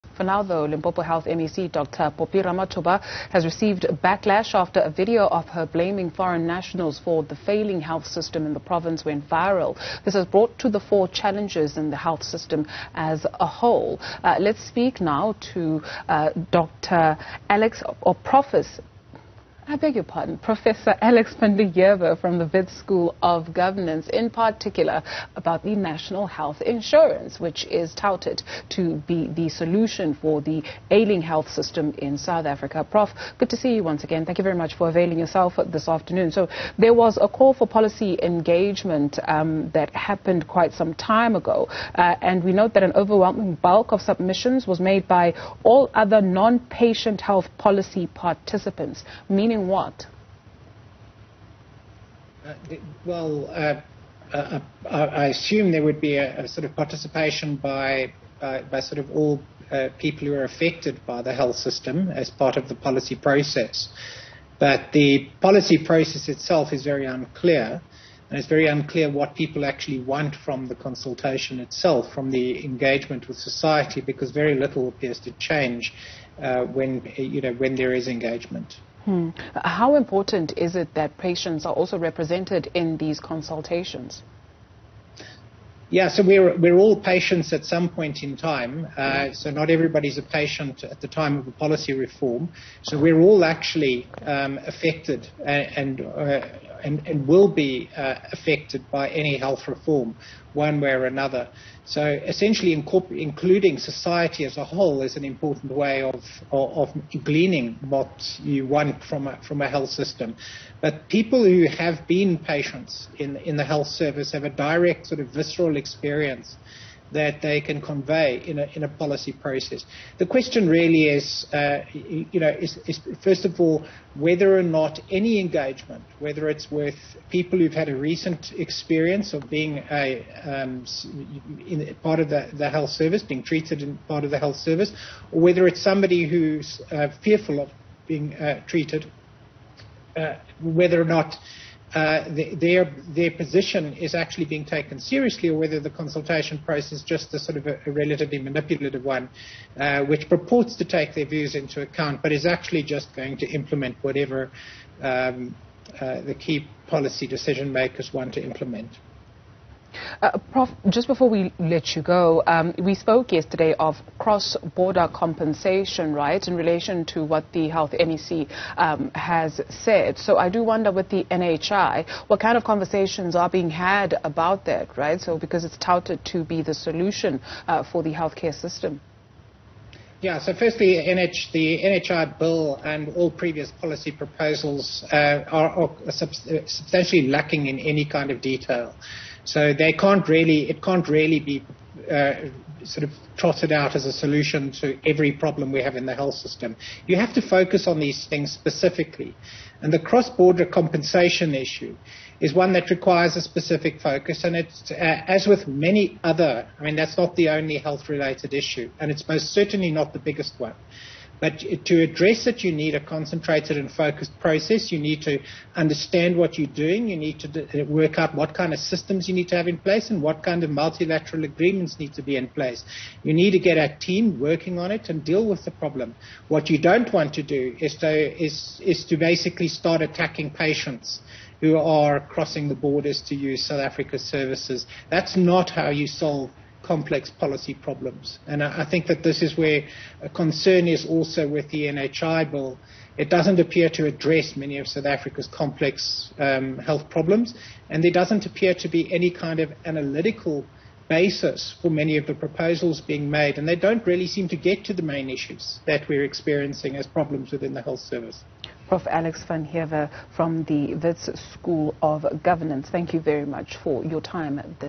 Limpopo Health MEC receives backlash
Limpopo Health MEC, Dr Phophi Ramathuba, has received a backlash after a video of her went viral, where she is seen blaming a foreign national for the failing health system in the province.

Professor Alex van den Heever of Wits School of Governance weighs in.

Tune into Newzroom Afrika channel 405 for more.